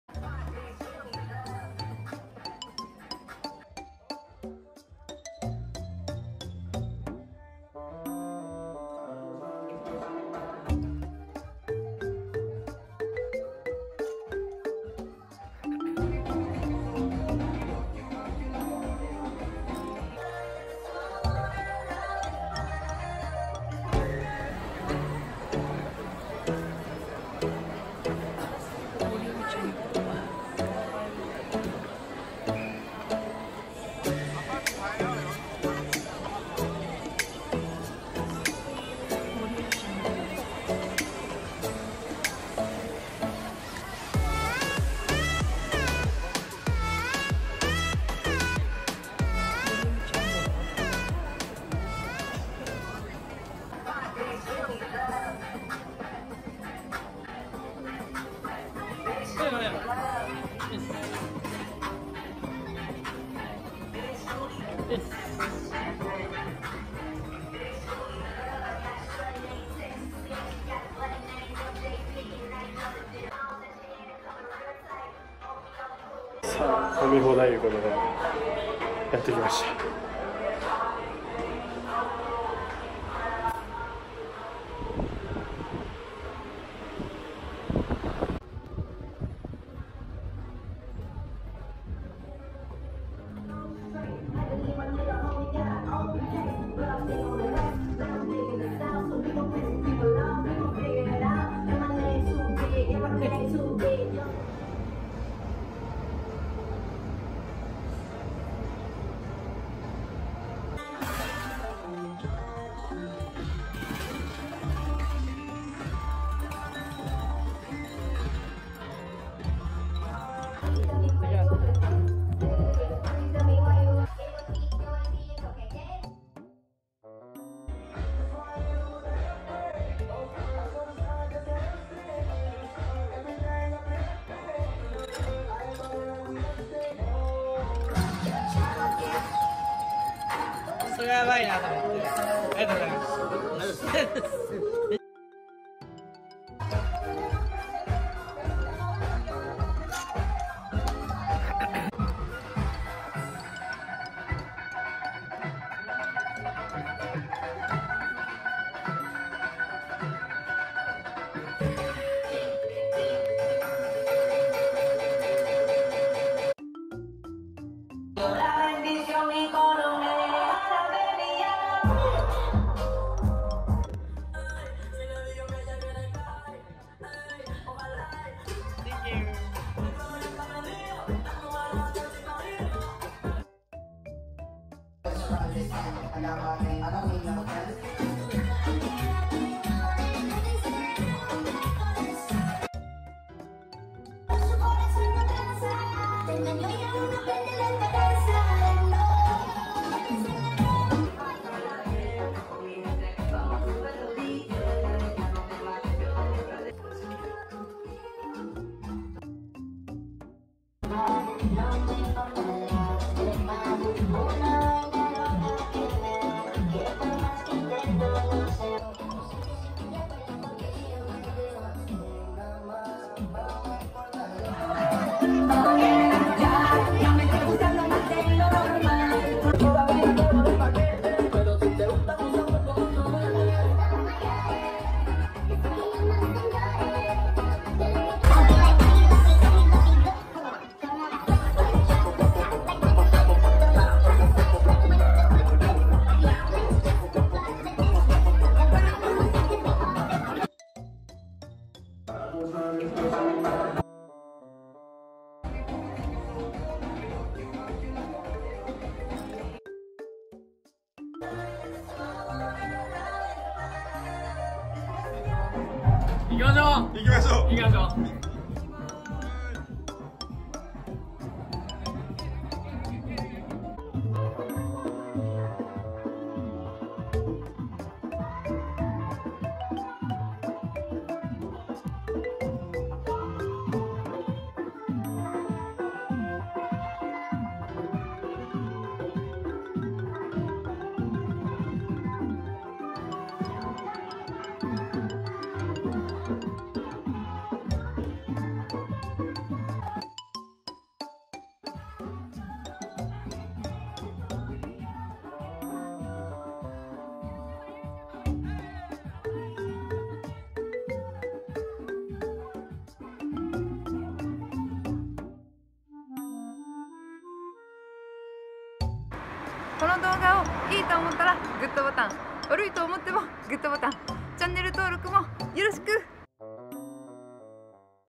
【 5時間 1000円で飲み放題】《一切ぼったくり追金なし》何杯飲めるか？ Club Pure Osaka Nightlife All you can drink
場所は PURE OSAKA

大阪 心斎橋 宗衛門町
岡山ネタ中心にいろんなジャンルを投稿していきます！